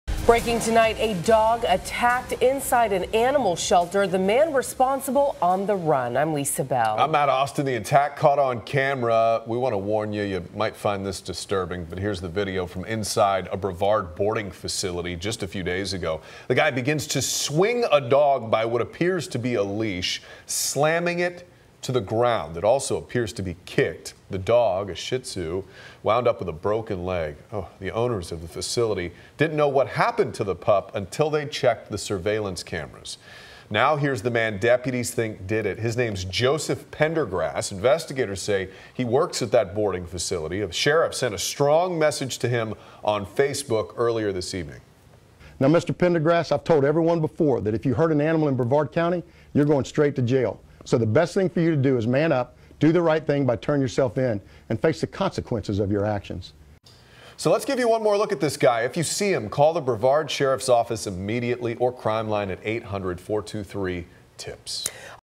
Man swung, slammed, kicked dog inside boarding facility, deputies say
A dog is attacked inside an animal shelter, and the man responsible is on the run.